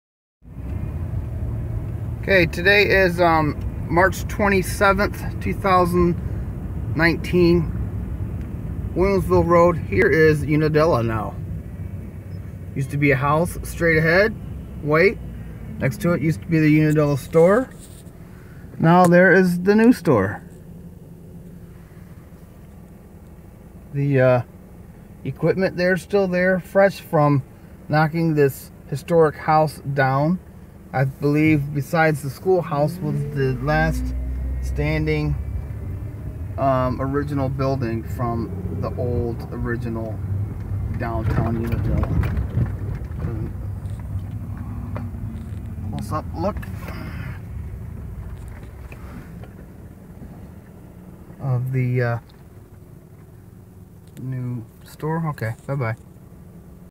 Unadilla, MI March 2019
The historic Unadilla Store burned down in Jan. 2015.
The white house was demolished earlier this month (March 2019).

In a related interesting story, the owner of the store won the Lotto!